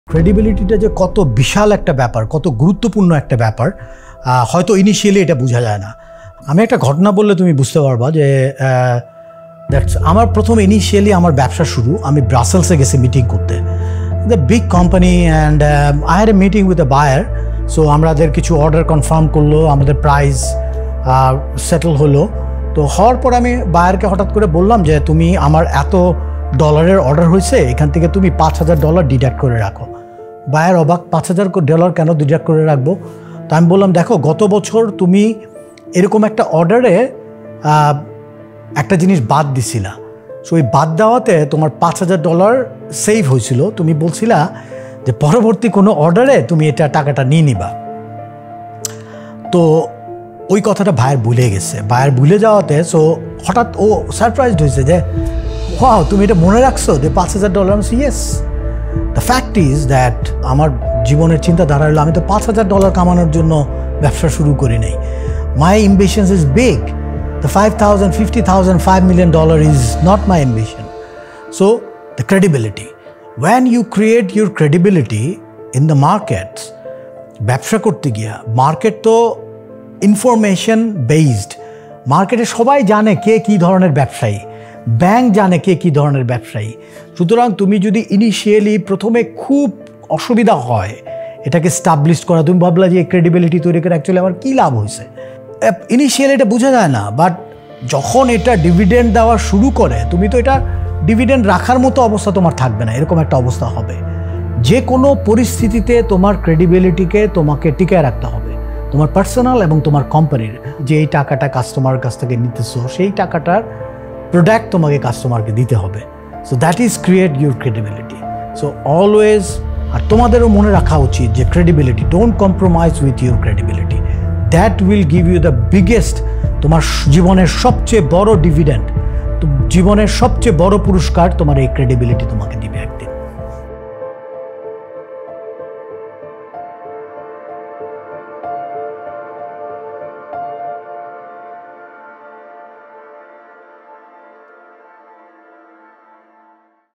Entrepreneur's Biggest Weapon | The Power of Credibility | Reaz Uddin Al-Mamoon
এই ভিডিও থেকে আমরা একজন Entrepreneur-এর করণীয় সম্পর্কে জানতে পারি। এরই সাথে যে কোন উদ্যোগে Credibility কত বেশি জরুরী সেটিও বলা হয়েছে ভিডিওটিতে।

Instructor: Reaz Uddin Al-Mamoon, Founder & Managing Director Epyllion Group

00:09 Important of Credibility
01:59 Don’t Compromise Your Credibility